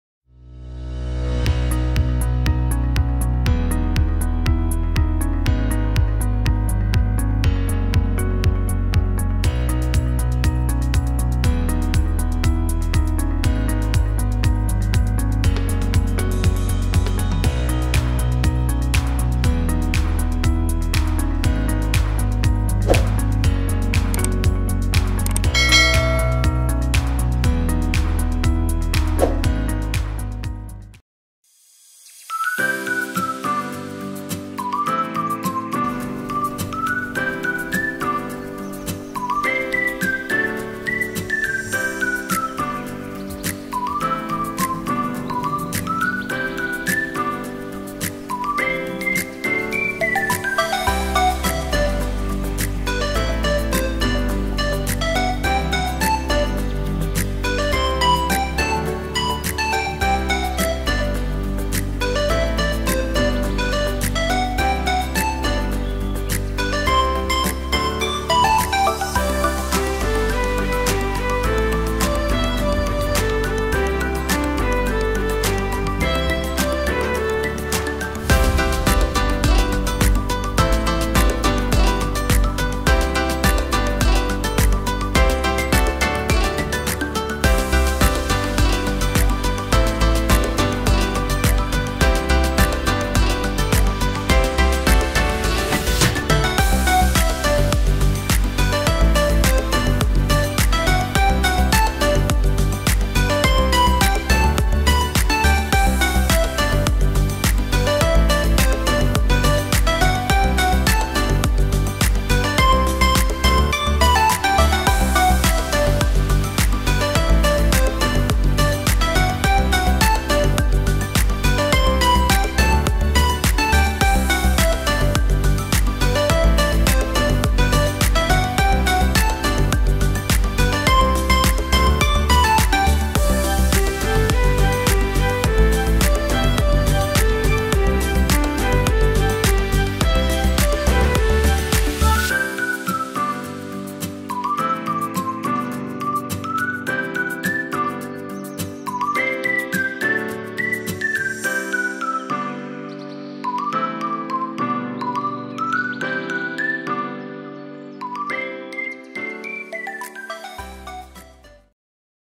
BACKGROUND KELAS VIRTUAL | BACKGROUND PEMBELAJARAN DARING | PART 3
Berikut adalah 5 background kelas virtual keren yang dapat digunakan sebagai background saat zoom atau meeting online dengan siswa.
5 Background ini sangat direkomendasikan untuk dipakai dan bebas copyright...
Background berupa ruangan kelas yang dapat juga digunakan sebagai background video pembelajaran.

Semoga bermanfaat 🙏

Sumber : SlidesGo